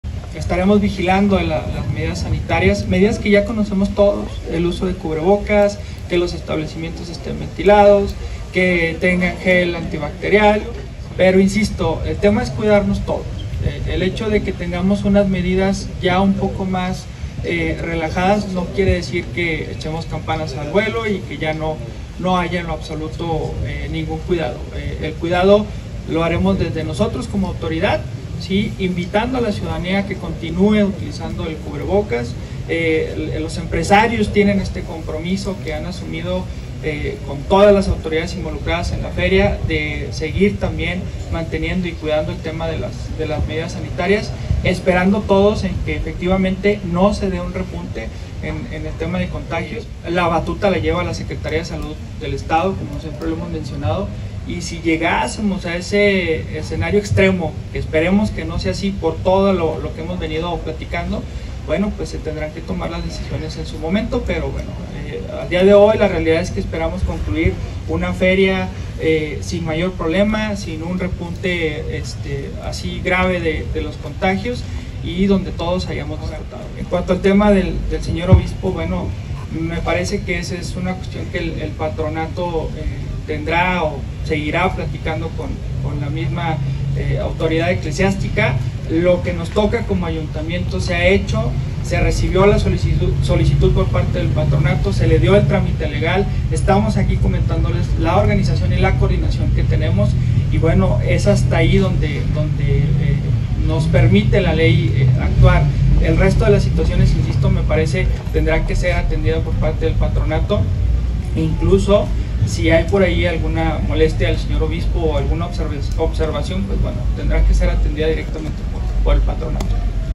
No se desea, pero tampoco se descarta suspender Feria si durante la misma se incrementan contagios.
Tras hacer un llamado a los feriantes a seguirnos cuidando, utilizar el cubrebocas, gel antibacterial y, en la medida de lo posible, guardar la sana distancia al asistir a eventos de la Feria Nacional de San Marcos, el Secretario del Ayuntamiento, Jaime Gerardo Beltrán Martínez no descartó que la fiesta pueda suspenderse en caso de un incremento desmedido de contagios.
En conferencia de prensa, reconoció que la decisión debería tomarla, en su caso, la Secretaría de Salud, con quienes se trabajará en estrecha coordinación.
Cuestionado sobre los señalamientos del Obispo de la Diócesis de Aguascalientes, Juan Espinoza, quien ha pedido en varias ocasiones a los aguscalentenses no distraerse la noche del Viernes Santo asistiendo a la verbena, Beltrán Martínez consideró que el buscar un acercamiento con la Iglesia Católica corresponde a la autoridad del Patronato de la Feria.